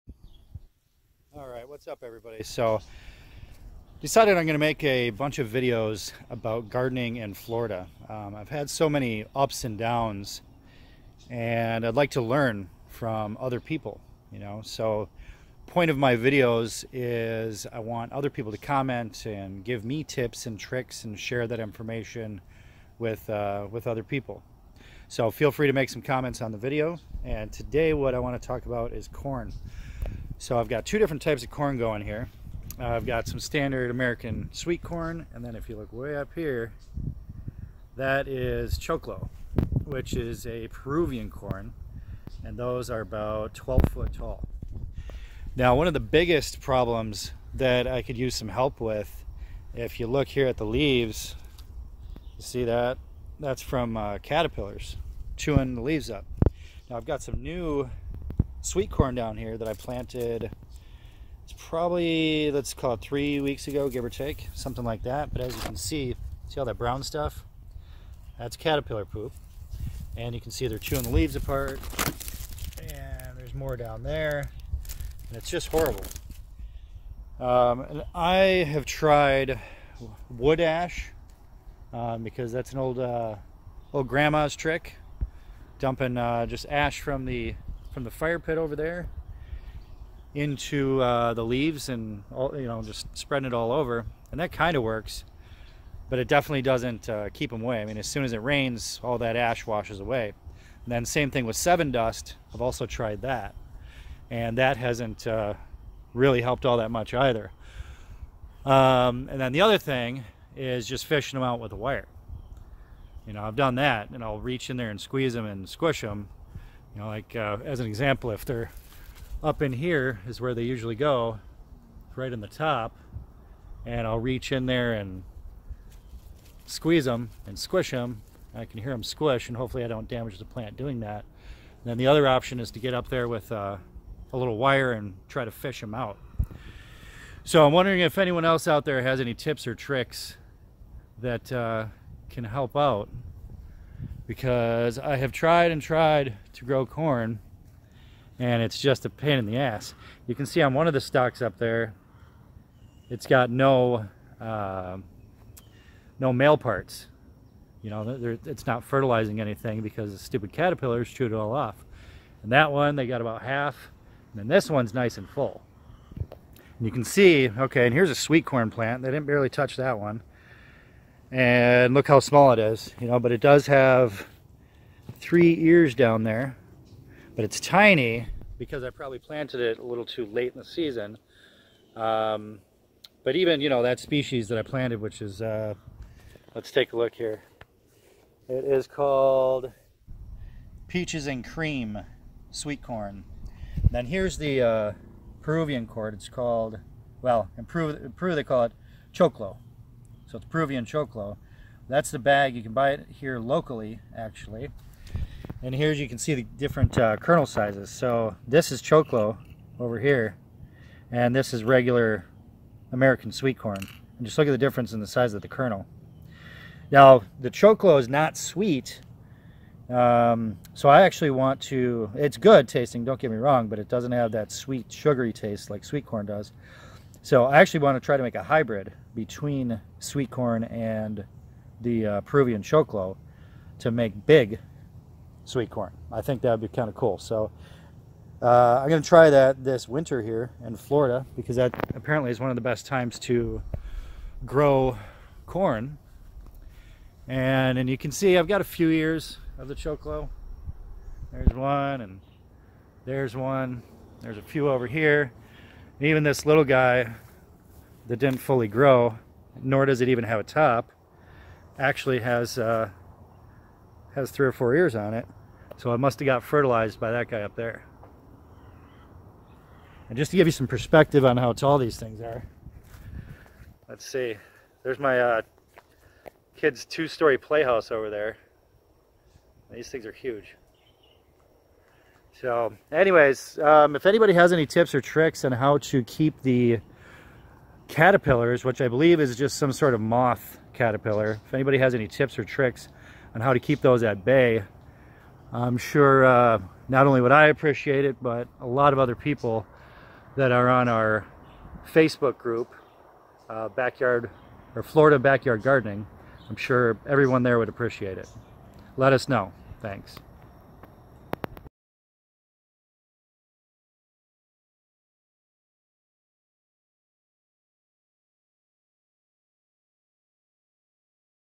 How to Grow Corn in Florida?
Growing corn in Florida is not easy, what's the best ways... Best Soil, Bug prevention, Time of year etc... Share your tips with us.